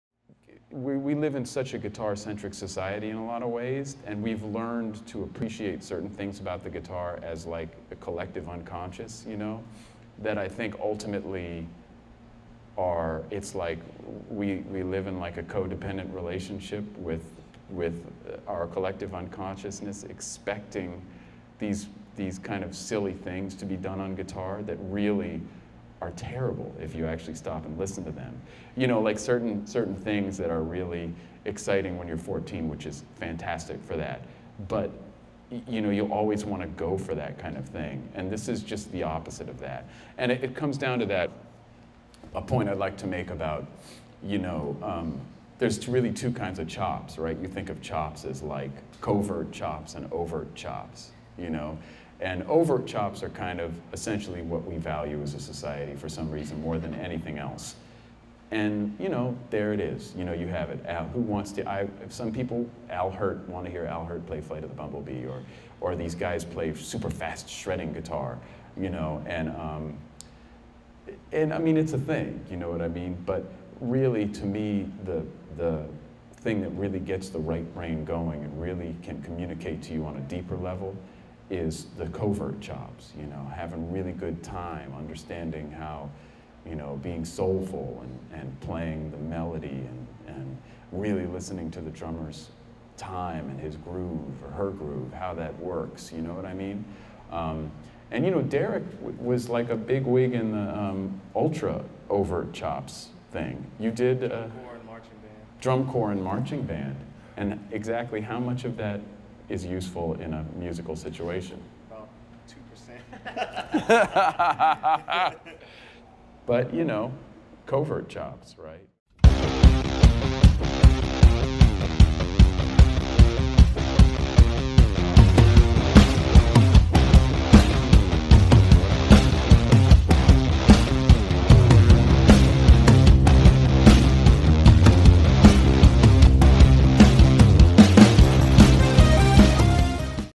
We Love Guitar Nerds - Charlie Hunter's Overt Chops vs. Covert Chops
Overt Chops vs. Covert Chops? Do you know the difference?  In praise of "overt chops"!!